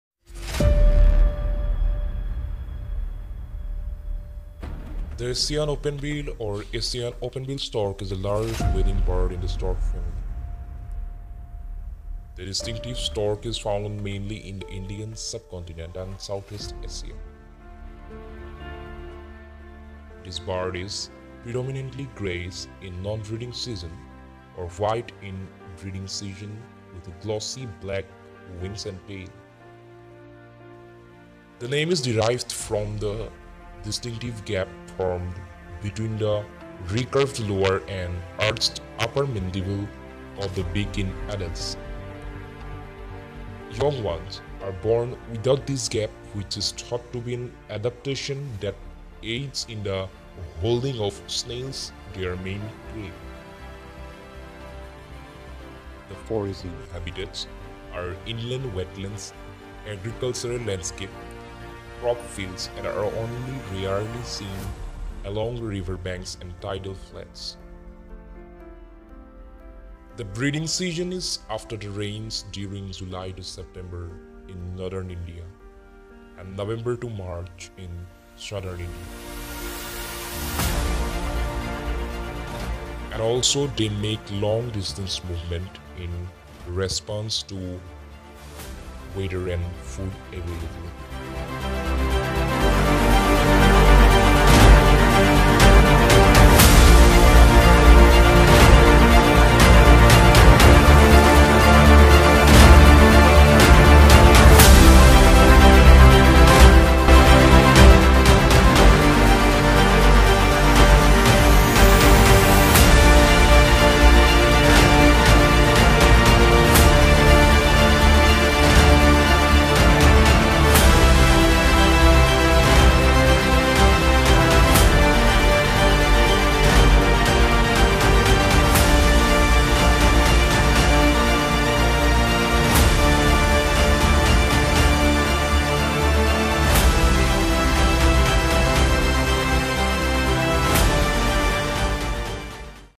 ASIAN OPENBILL || SHORT DOCUMENTARY FILM || WILDLIFE
A short documentary on Asian openbill stork.

I wish you will love it ❤️

Device used :

@canonindiapvtltd 200d (with 55-250mm )

all the editings done in mobile ( Motorola g40 fusion )

Apps :

1. kinemaster
2. lexis audio editor
3. Google photos ( for stabilization)